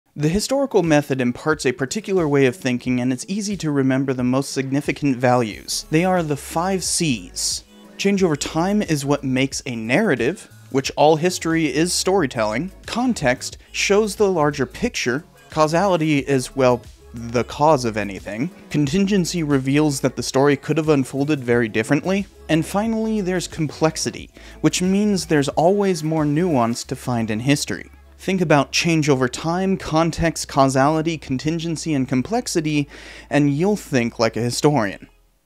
Thinking historically with the 5 Cs
The historical method imparts a particular way of thinking, and it's easy to remember the most significant values.  They are the 5 Cs.  Change over time is what makes a narrative.  Context shows the larger picture.  Causality is, well, the cause of anything.  Contingency reveals that the story could've unfolded very differently.  Finally there's complexity, which means there's always more nuance to find in history.  Think about change over time, context, causality, contingency, and complexity and you'll think like a historian.